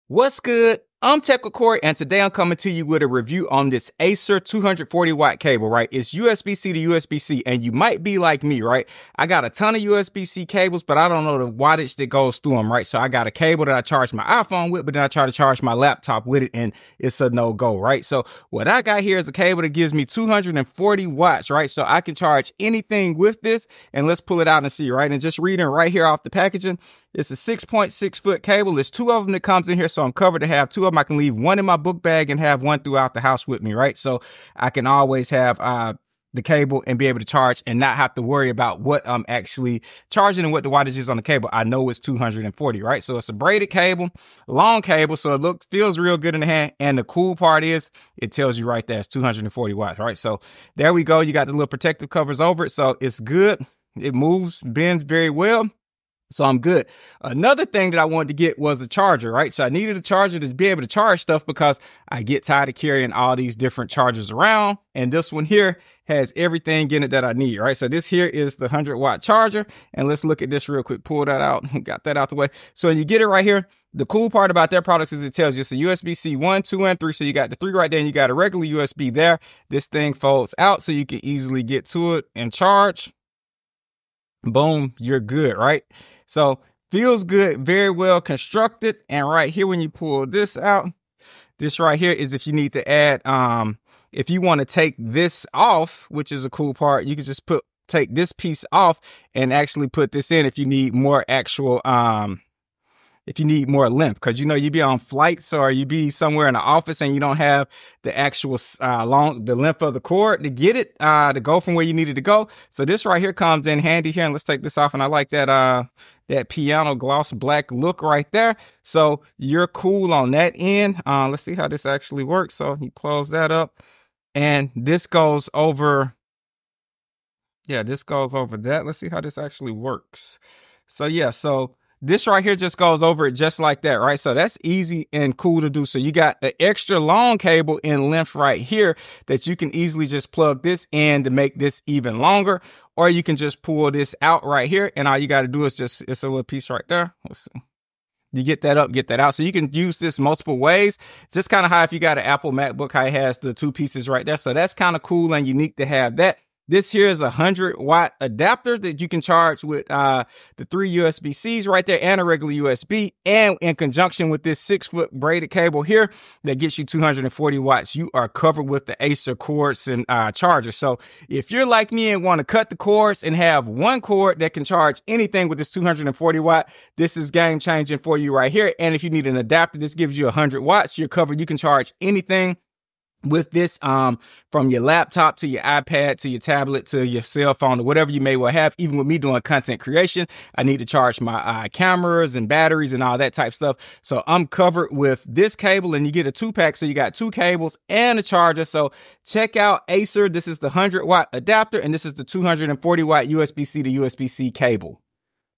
FASTEST 240W USB Type C Cable You Can Buy in 2025!
The Acer USB-C to USB-C cable 2-pack provides reliable power, delivering up to 240W or 100W for ultra-fast charging.These versatile cables support a range of power outputs, including 66W, 60W, 40W, and more, ensuring compatibility with various devices. With support for multiple fast charging protocols like PD and QC, your devices will charge quickly and efficiently. Crafted with durability in mind and featuring reversible connectors, these cables are perfect for laptops, smartphones, tablets, and more.

【Power All Your Devices】This 240W usb c cable backward compatible with 140W/ 100W/ 65W/ 60W/ 45W/ 20W devices. Ideal for fast charging high-performance devices such as laptops, tablets, and smartphones.
【240W Super Fast Charging】Acer usb c to usb c cable supports PD3.1 and QC4.0 fast charging with a max 240W output, which can charge the iPhone 16 from 0% to 71% in 30 minutes. No more time wasted on charging. 📌NOTE: The actual charging power depends on your device and charger.
【Smart Charging Technology】Built-in E-marker chip that intelligently matches the necessary current and protection of your devices. Giving you peace of mind while charging.
【Durable and Stylish Design】Our nylon braided fast charging cable with reinforced joints provides long-lasting, tangle-free performance. The strain relief design and solid aluminum shield make the cable durable and reliable. Perfect for daily use and heavy-duty charging.
【Versatile Length Options】Available in 3.3ft, 6.6ft, and 10ft lengths to accommodate various usage scenarios, from compact setups to extended reach needs.